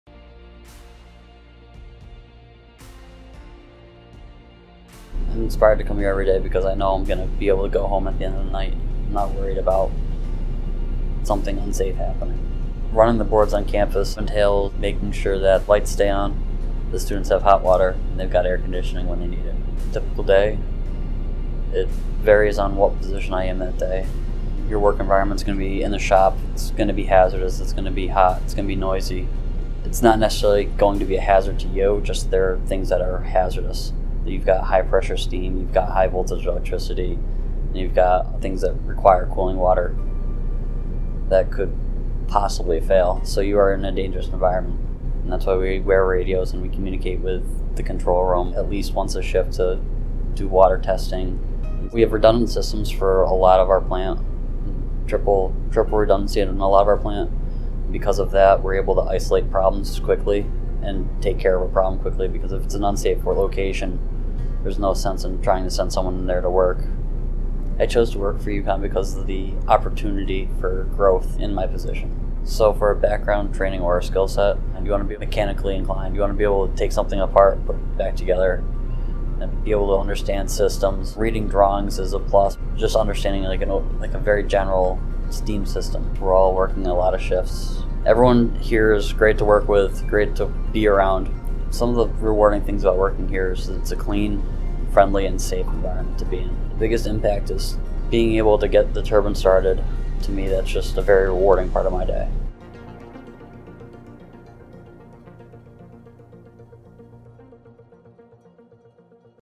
State of Connecticut | University of Connecticut | Power Plant Operator Jobs
Watch University of Connecticut Power Plant Operators talk about their jobs and day-to-day activities to ensure safe operations in the production and distribution of energy.